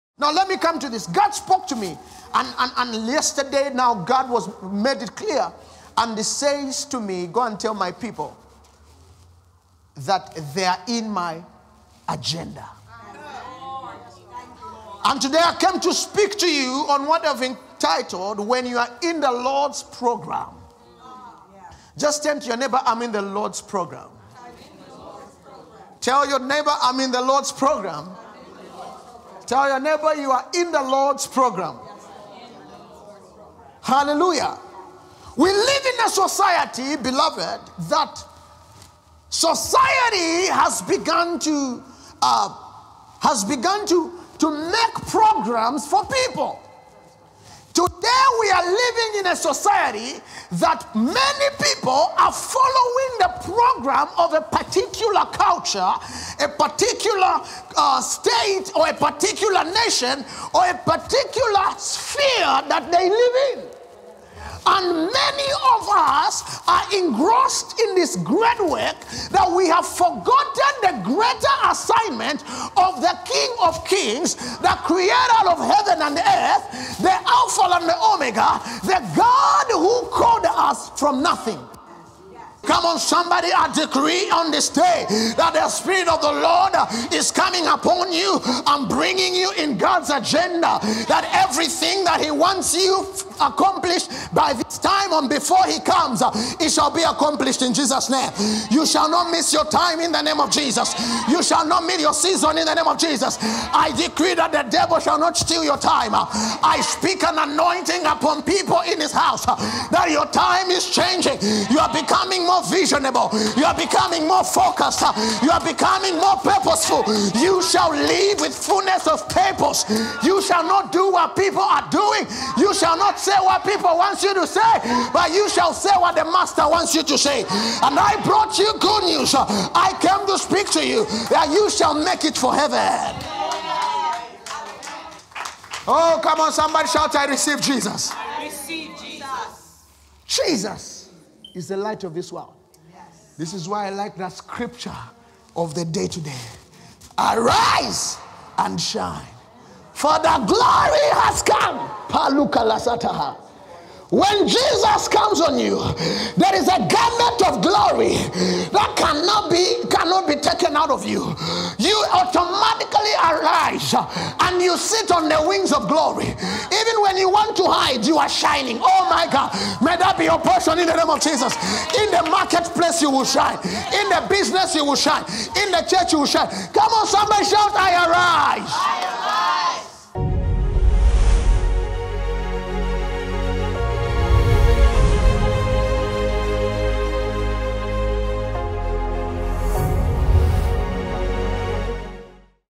2018 Spring Revival — Prophet Garry Bwalya
You are in God's agenda! Listen to these excerpts from the 2018 Spring Revival with speaker Prophet-Garry Bwalya!

Love. Connect. Worship. Grow. Serve. Go.